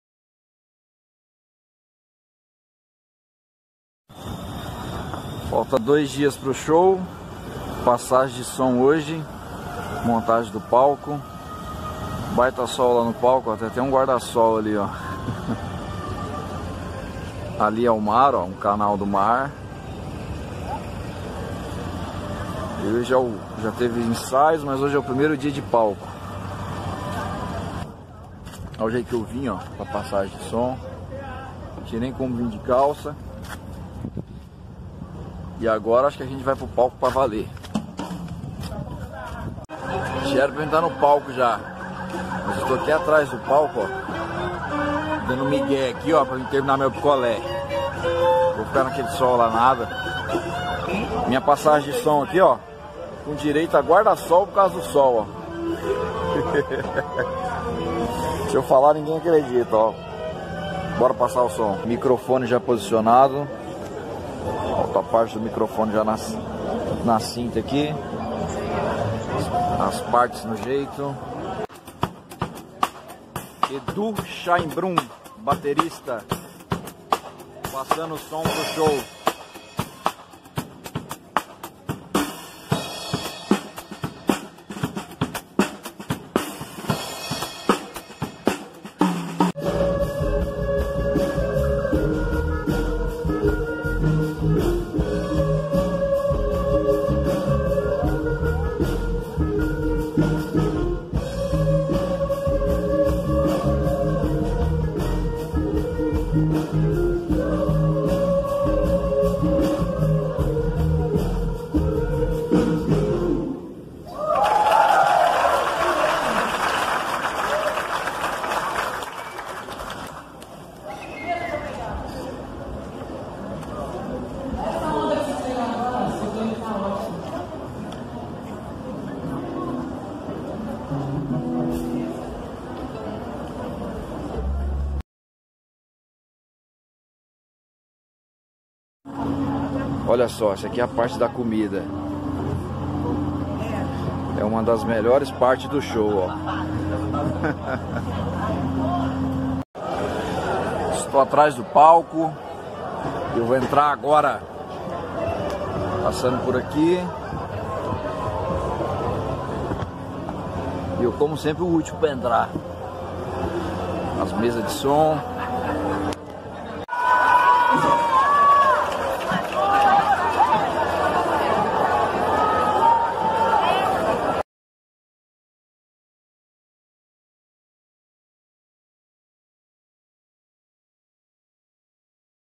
Bastidores - Show Vanessa da Mata
Show com a participação do vencedor do The Voice Kids 2019